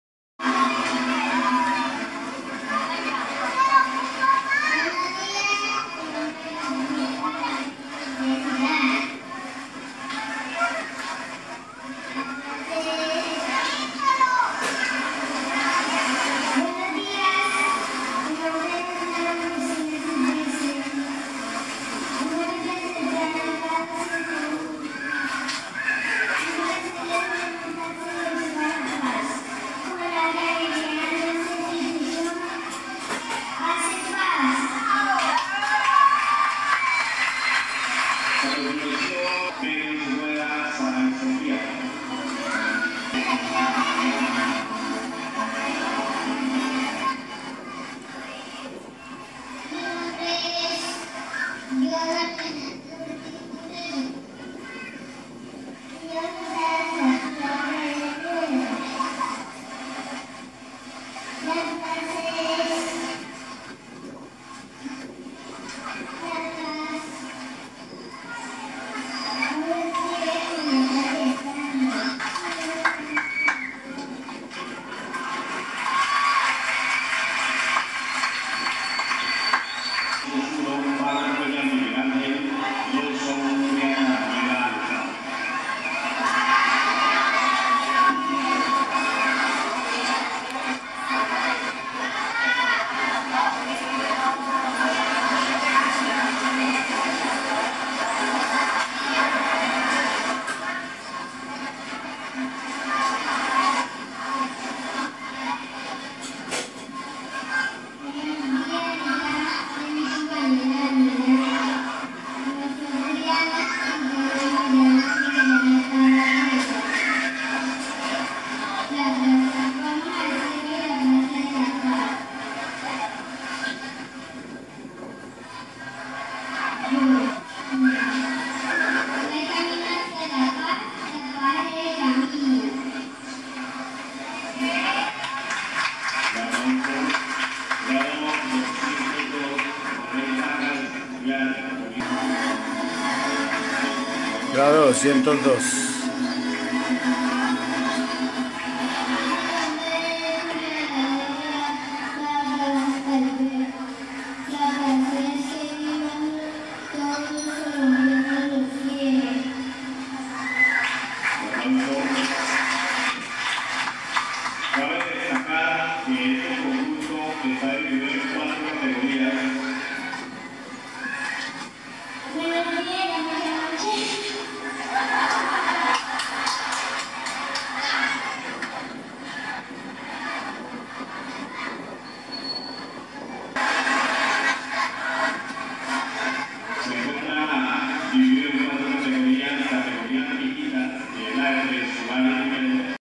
Desfile de gala de los llaneritos de primerito y segundo del Santa Teresa de Pachaquiaro.
02 agosto 2.016